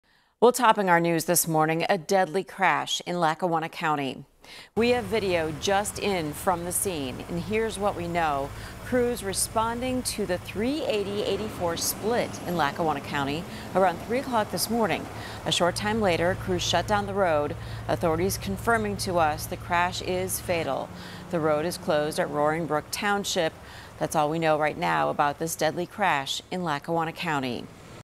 One person is dead after an early morning crash in Lackawanna County
Crews called to the Interstate for an early morning crash in Lackawanna County ii